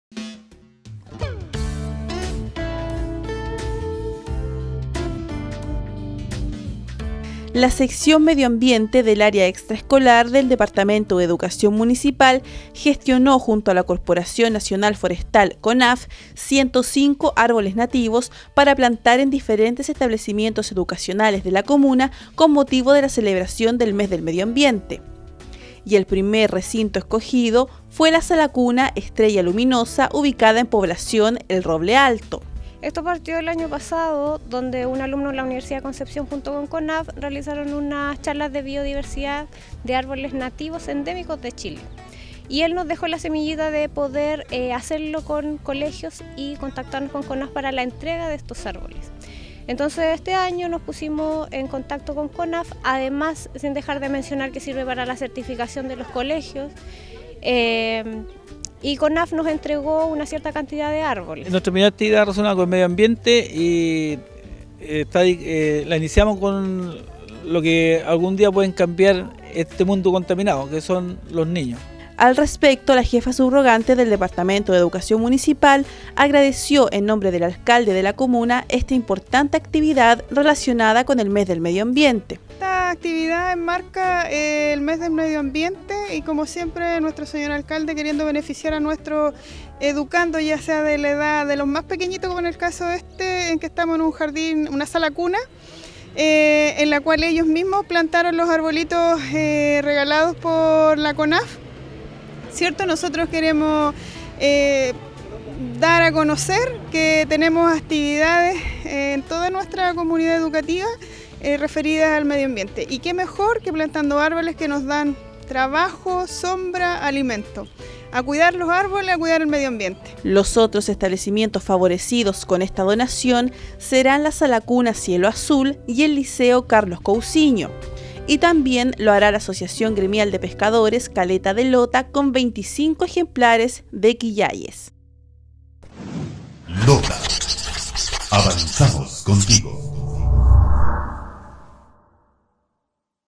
Plantación de árboles nativos en establecimientos educacionales
La Sección Medio Ambiente del Área Extraescolar del Departamento de Educación Municipal, gestionó junto a la Corporación Nacional Forestal (Conaf), 105 árboles nativos para plantar en diferentes establecimientos educacionales de la comuna, con motivo de la celebración del mes del medio ambiente. Y el primer recinto escogido fue la Sala Cuna Estrella Luminosa, ubicada en Población El Roble Alto.